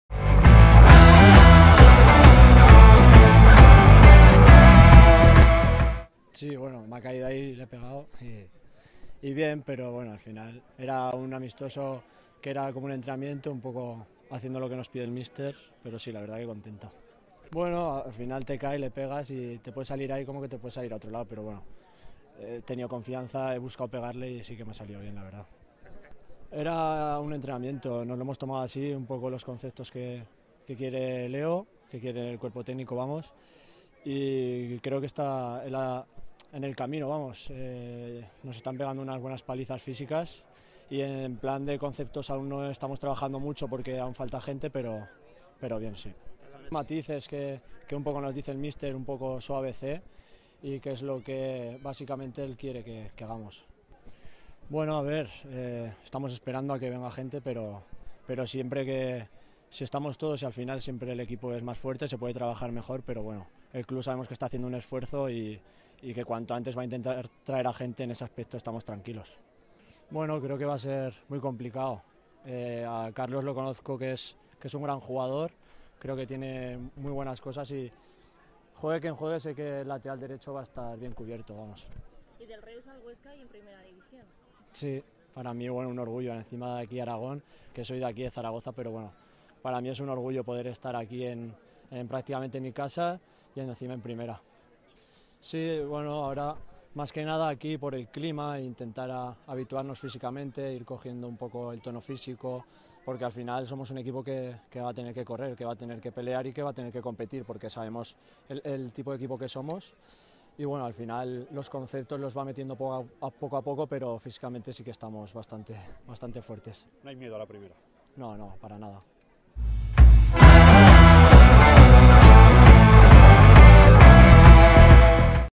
SD Huesca | Jorge Miramón habla tras el SD Huesca (5-0) Monzón ·14/07/2018·
La Sd Huesca disputó su primer amistoso de pretemporada ante un equipo de Tercera División. El lateral diestro, Miramón, dejó sus impresiones al finalizar.